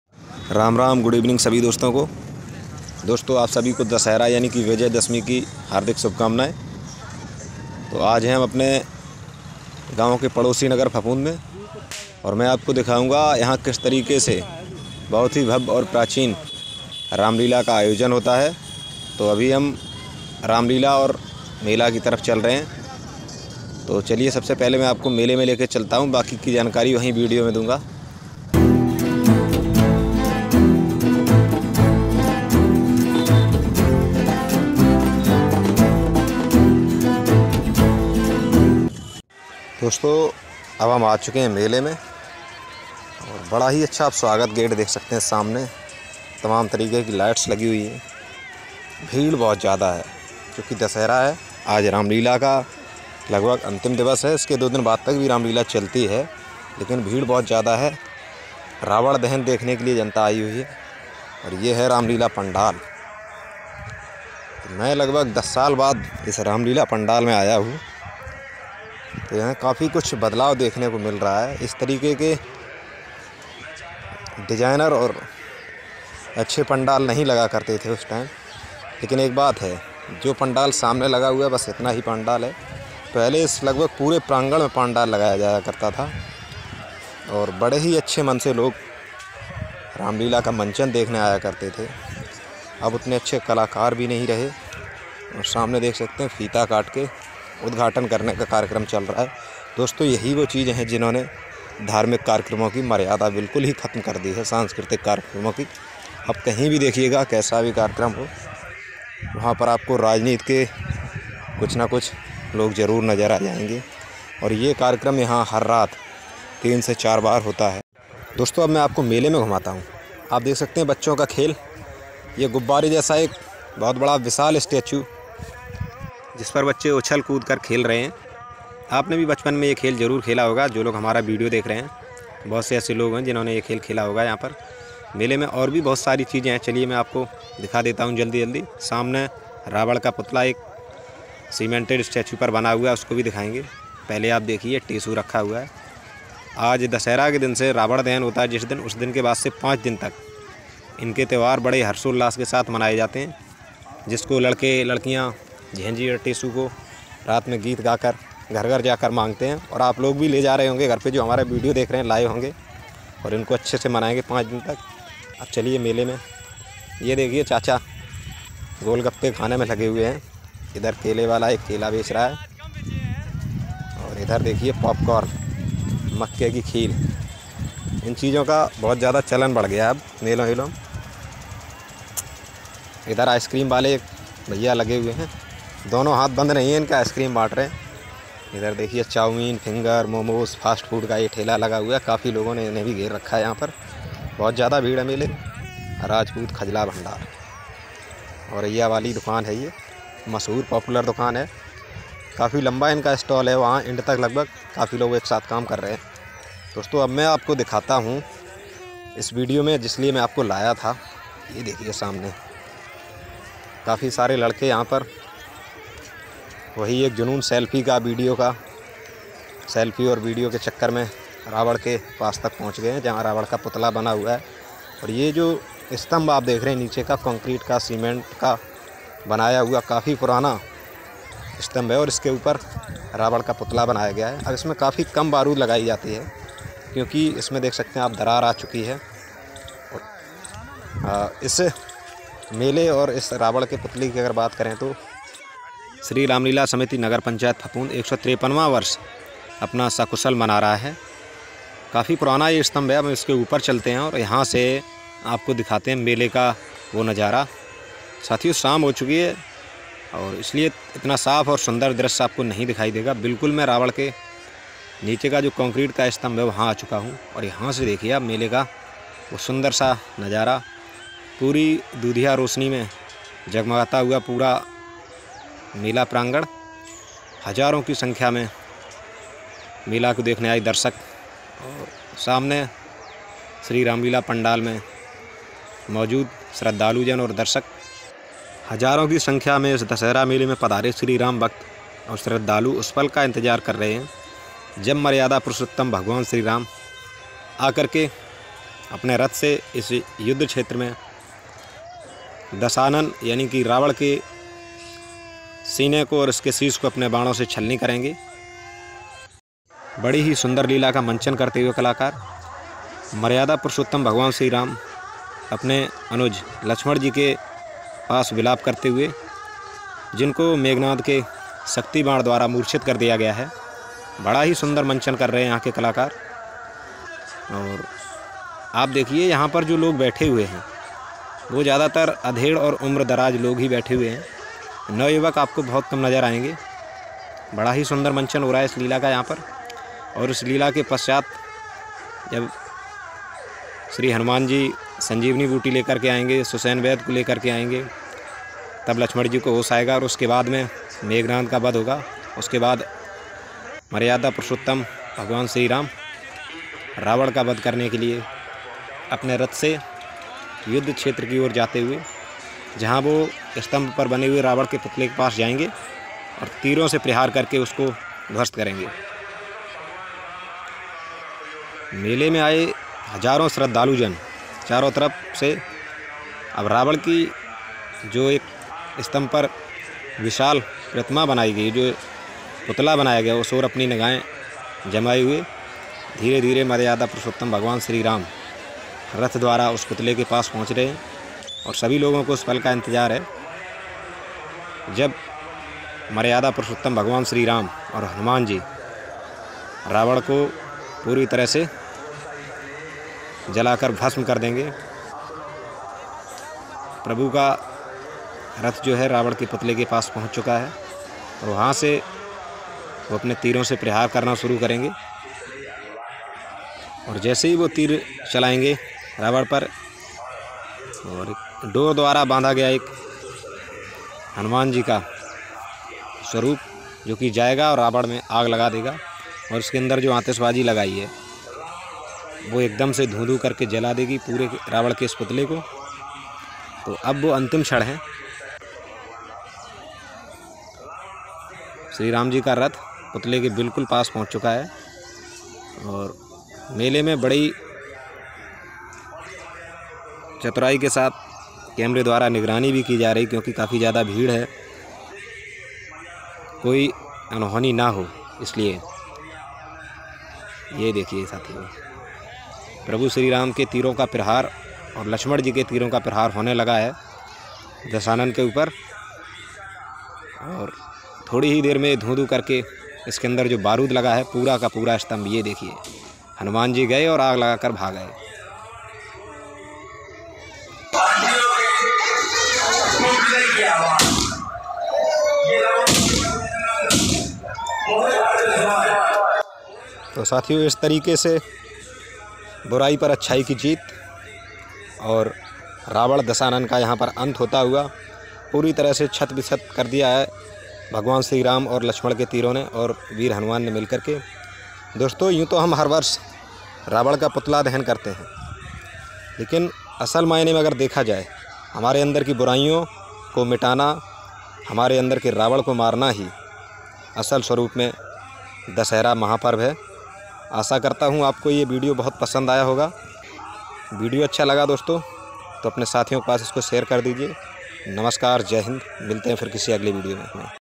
153 वां रामलीला महोत्सव और विशाल रावण का वध || mela vlog video||Phaphund ki Ramlila
असत्य पर सत्य की विजय के महापर्व दशहरा की आप सभी को हार्दिक शुभकामनाएं एवं बधाई दोस्तो आज हम अपने गांव के करीबी शहर फफूंद शरीफ में अति प्राचीन काल से चली आ रही 153 वर्ष पुरानी रामलीला दशहरा मेला और रावन के वध का सुंदर और विहंगम दृश्य देखने आए हैं वीडियो के माध्यम से आपको भी कुछ पल दिखाने की कोशिश की है आशा करता हूं वीडियो आपको पसंद आयेगा🙏